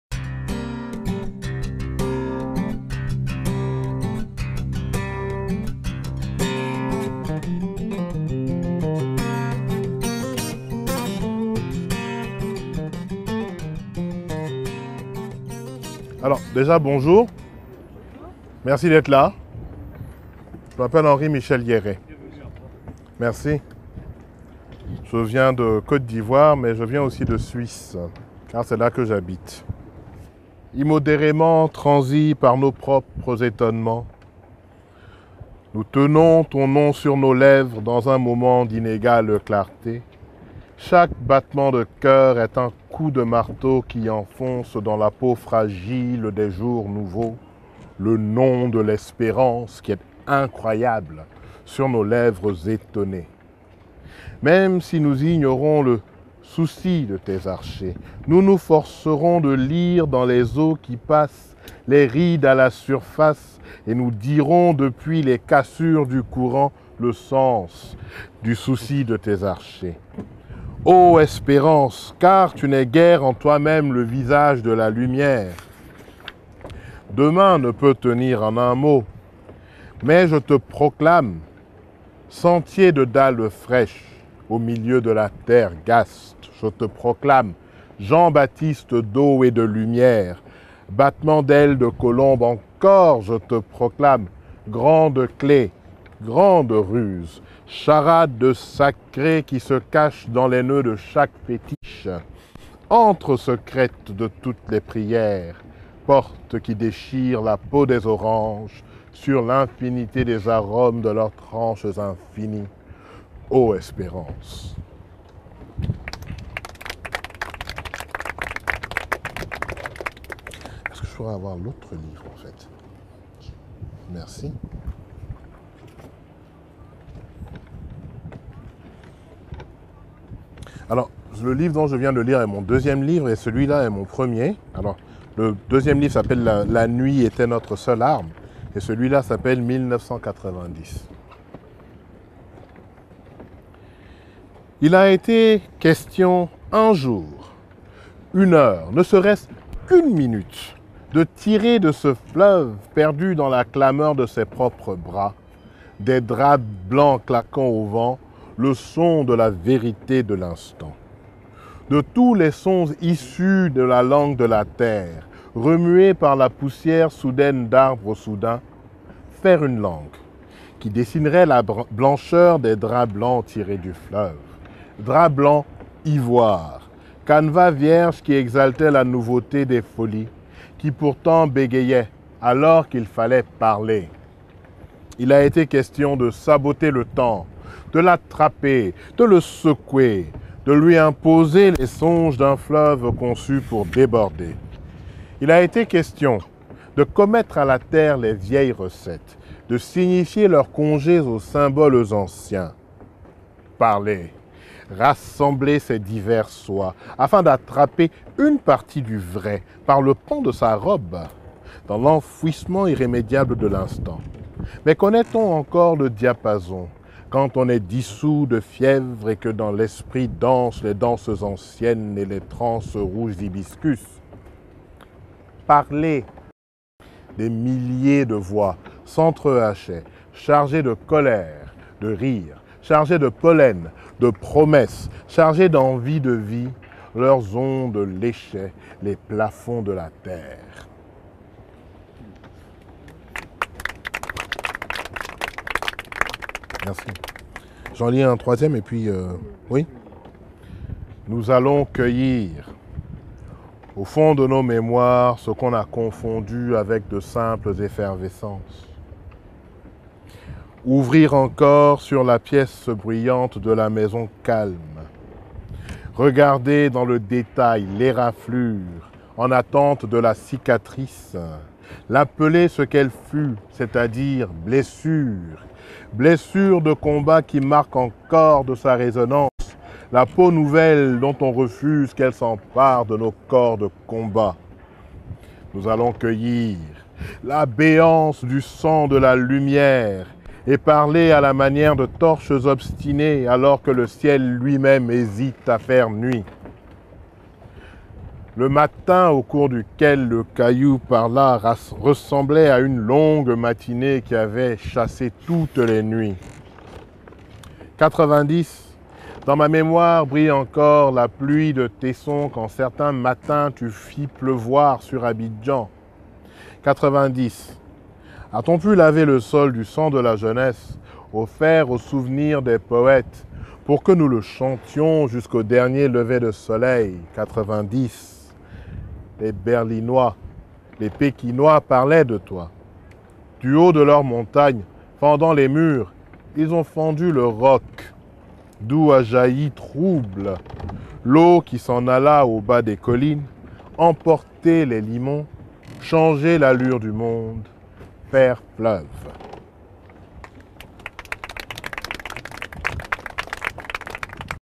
Festival Voix Vives 2018 : Henri Michel Yéré
Festival Voix Vives 2018
Lecture intimiste en mer : Henri Michel Yéré
Images et montage : Thibault Grasset - ITC Production